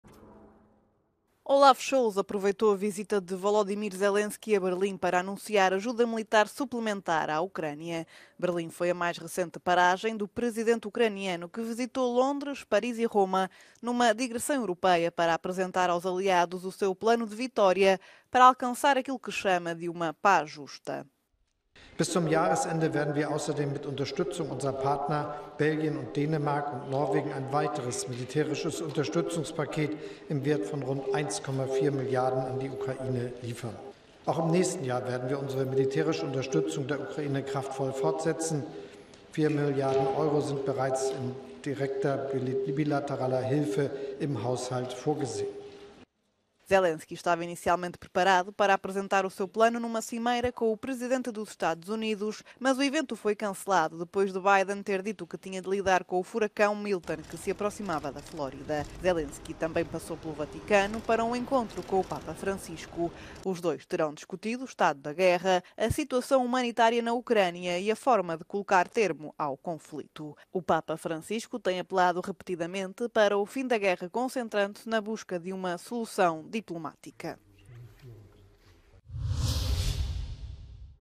Scholz anuncia novo pacote de ajuda militar à Ucrânia ao lado de Zelenskyy em Berlim
Chanceler alemão anunciou nova ajuda de 600 milhões para apoio militar para o país, além de um pacote de 1,4 mil milhões de euros, juntamente com alguns aliados ocidentais. O presidente ucraniano diz que o país “quer um fim justo e rápido para guerra”.